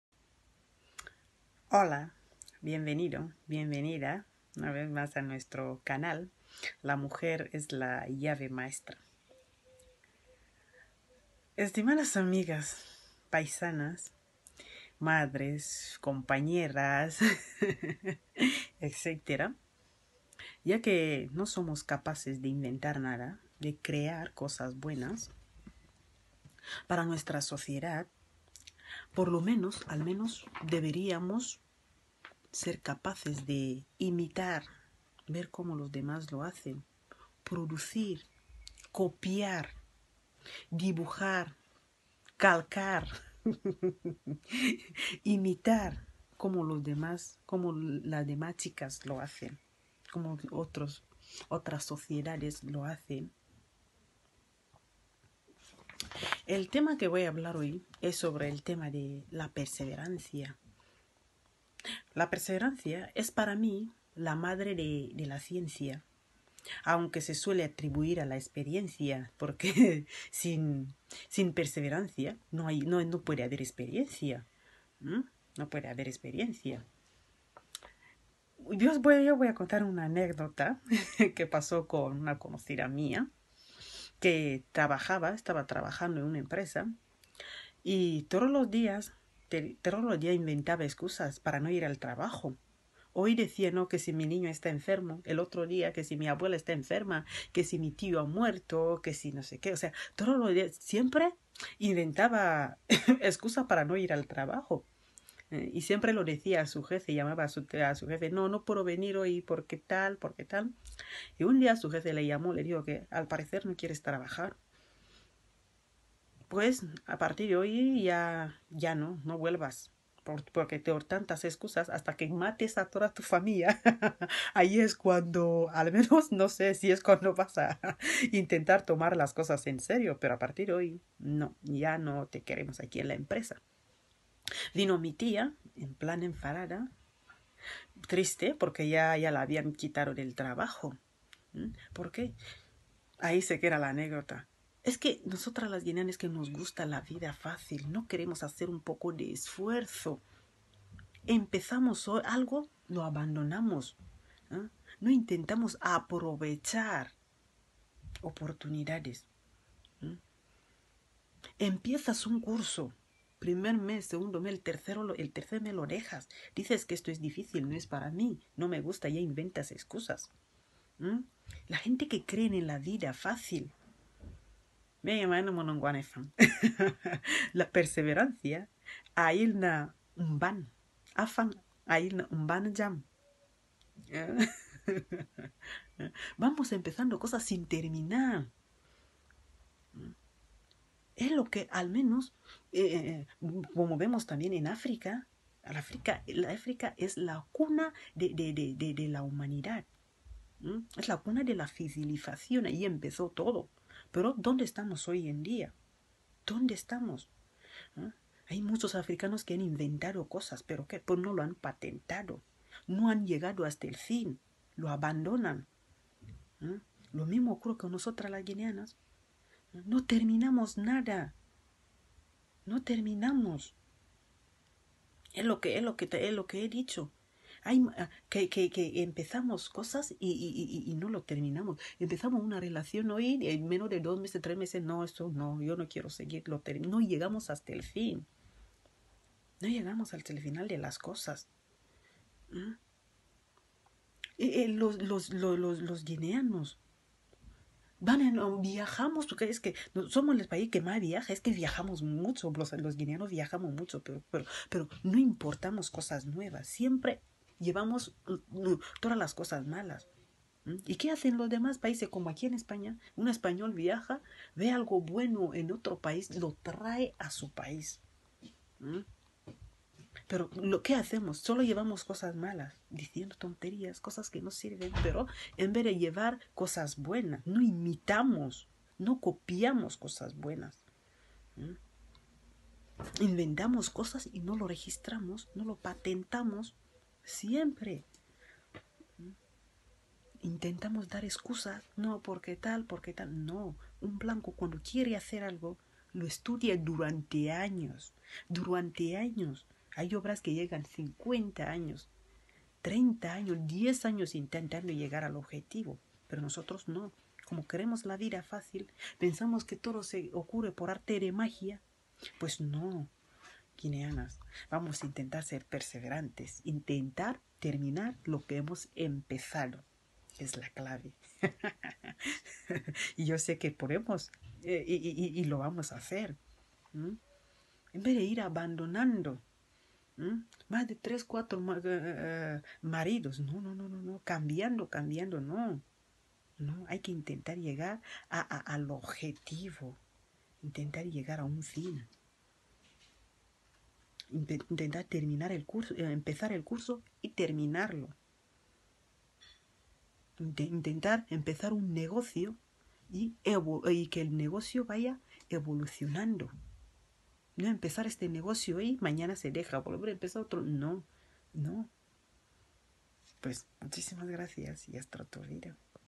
LA PERSEVERANCIA
Guineanas tenemos que ser perseverantes,cueste lo que cueste para llegar al objetivo.